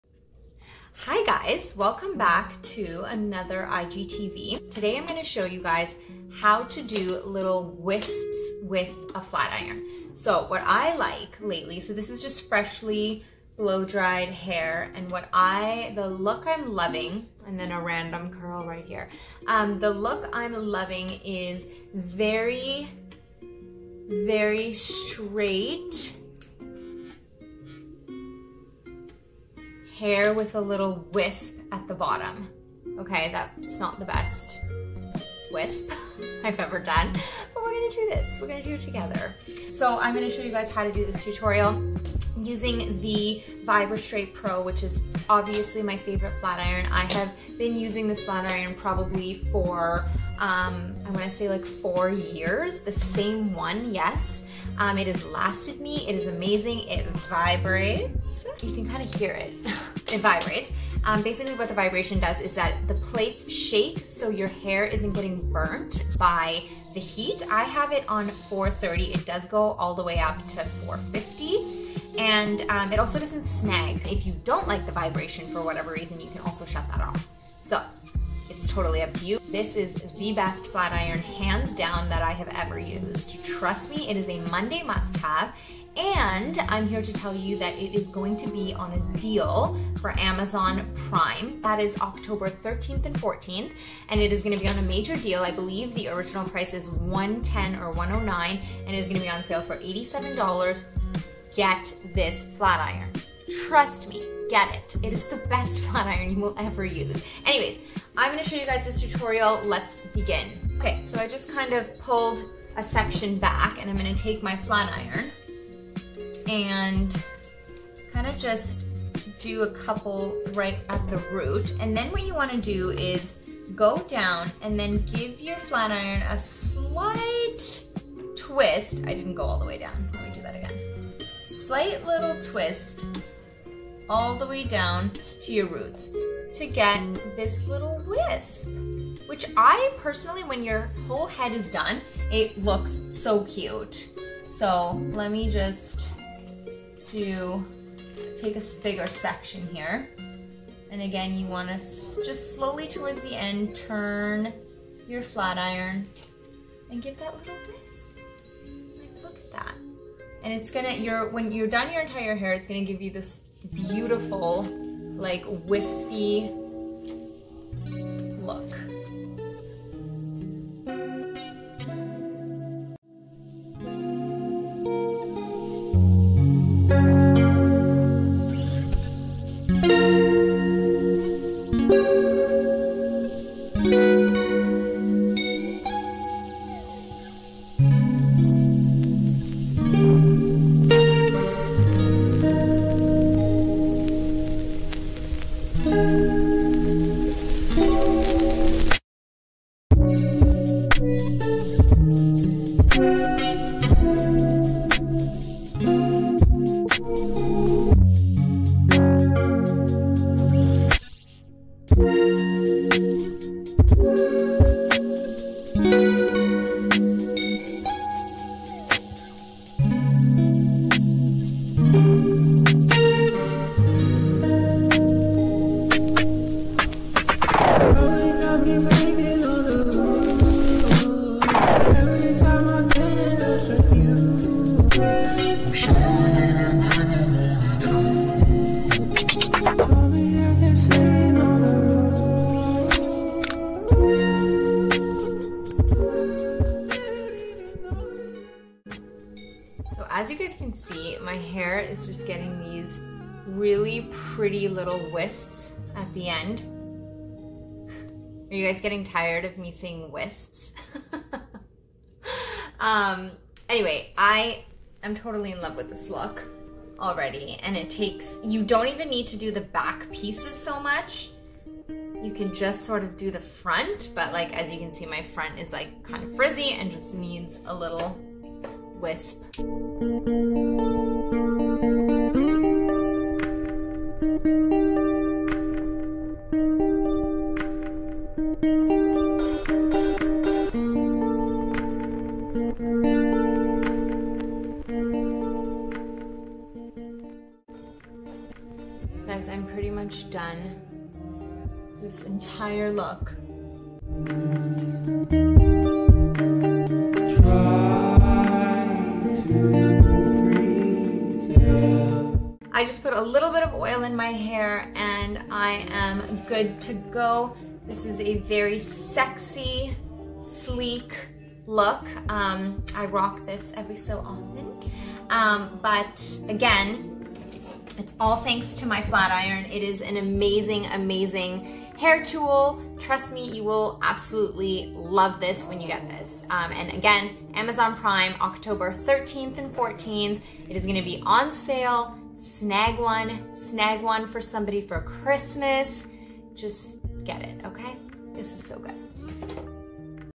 SLEEK AND SHINY STRAIGHTENING TUTORIAL| VIBRASTRAIT IRON| BEAUTOPIA HAIR
Sleek and shiny hair tutorial with the Vibrastrait PRO 1 inch

Featuring Kimberley Marquis @thechicconfidential